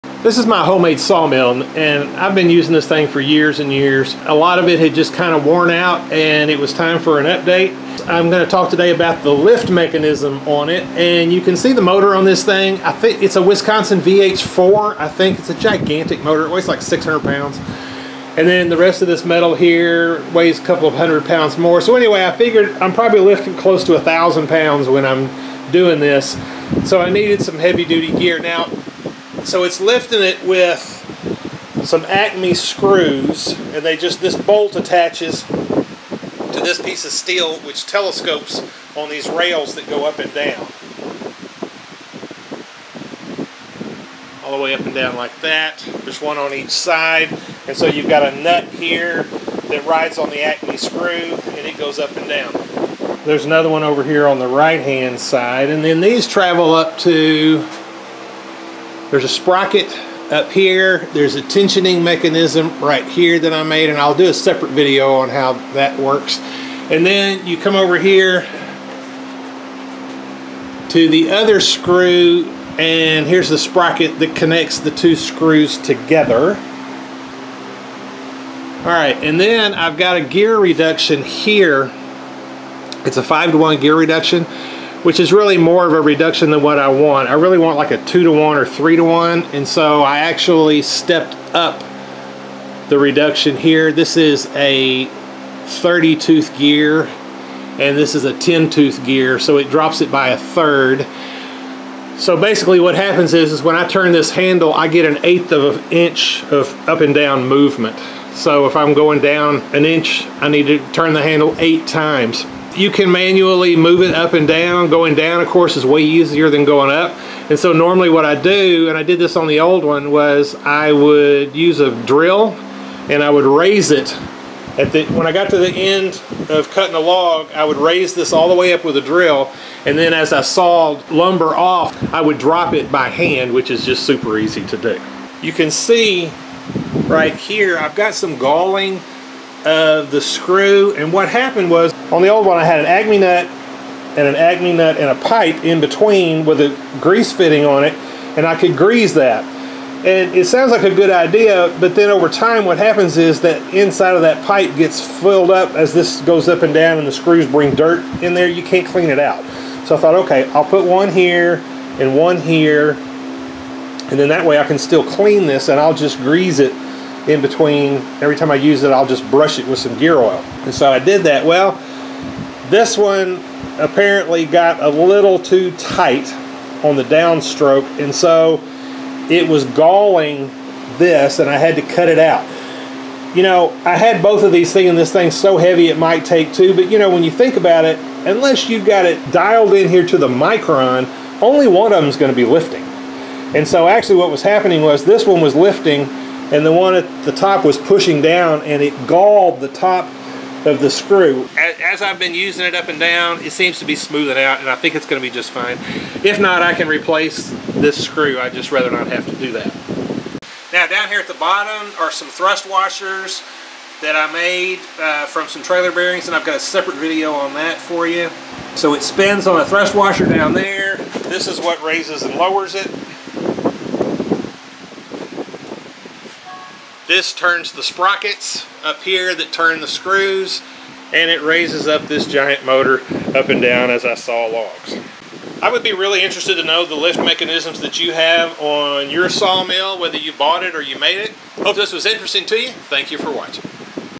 Sawmill ACME Lift System
How my ACME screw system is used to raise and lower my very heavy sawmill.